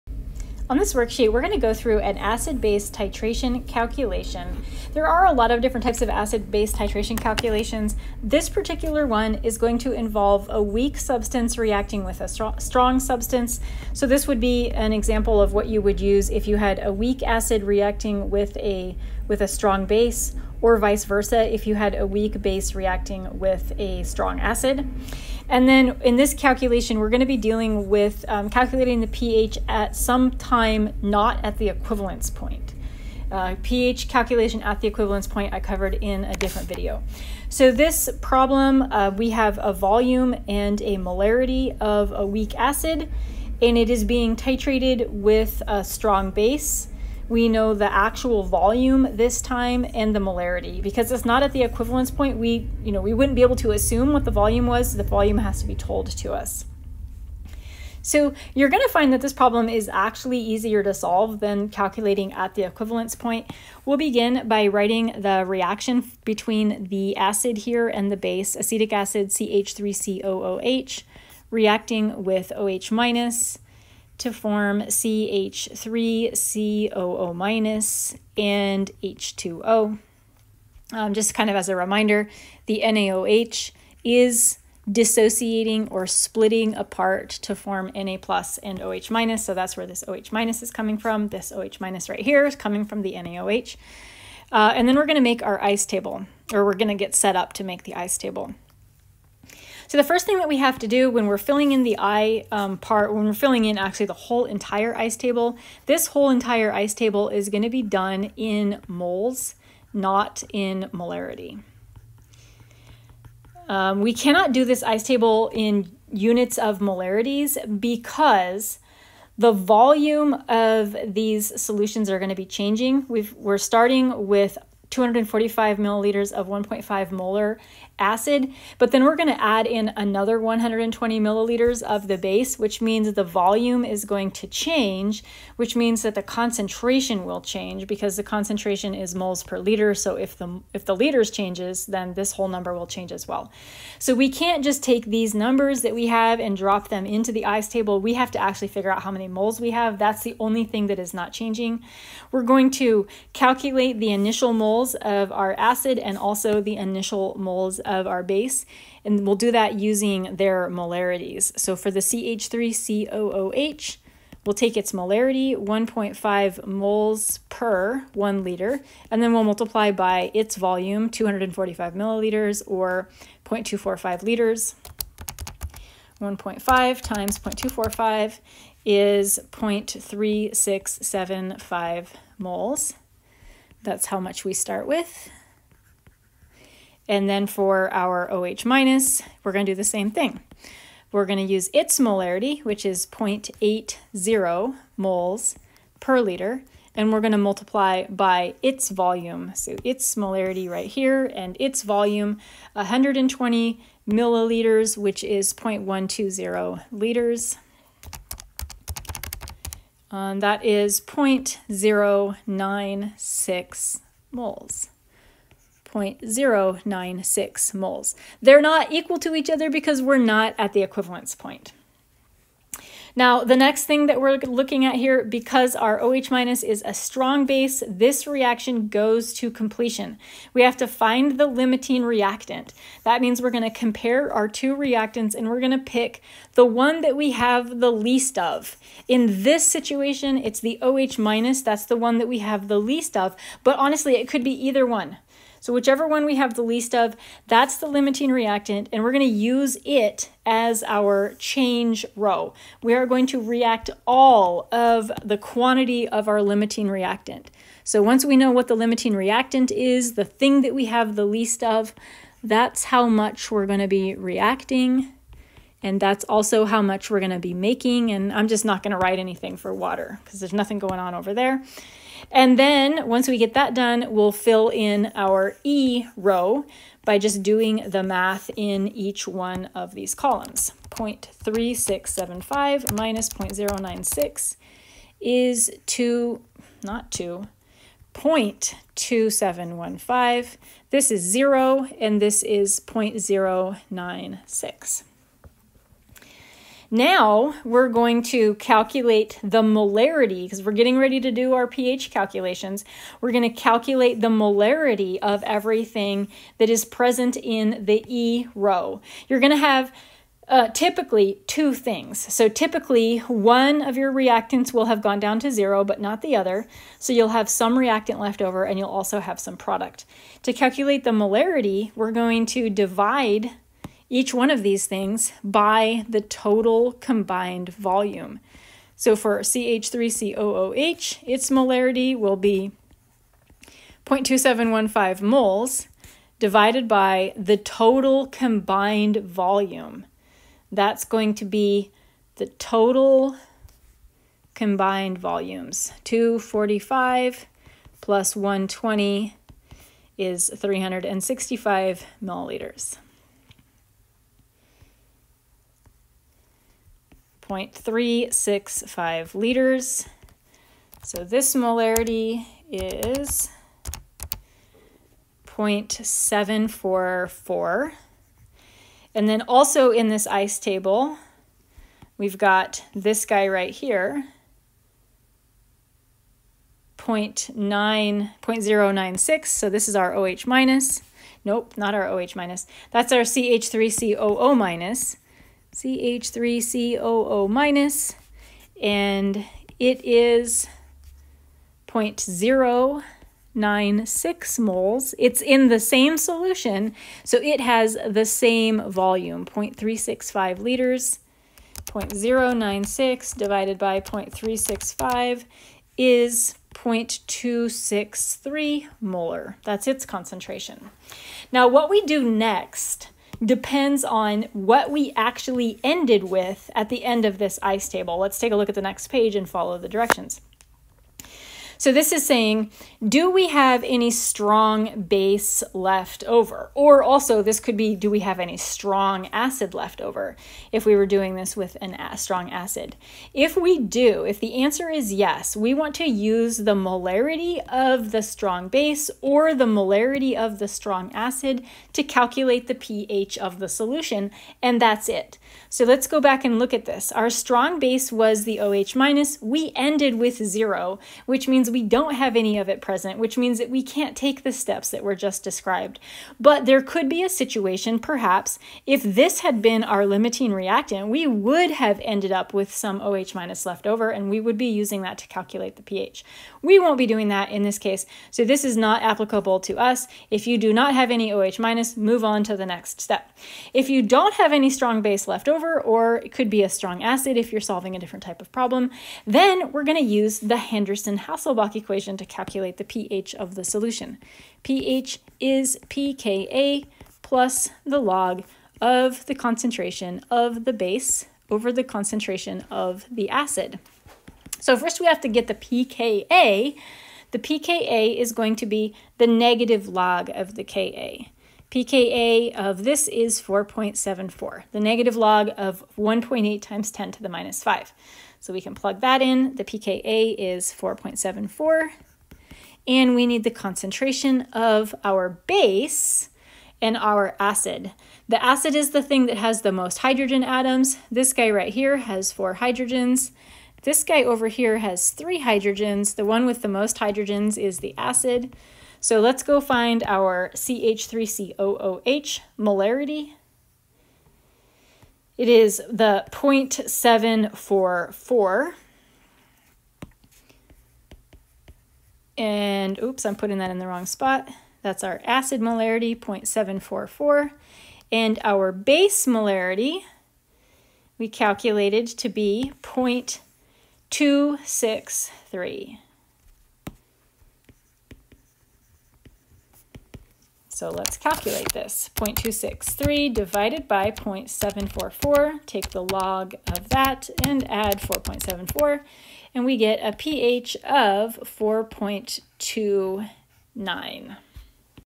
35b: Calculating the pH during a strong-weak titration
How to calculate the pH of solution during any point of the titration of a strong acid and weak base, or weak acid and strong base.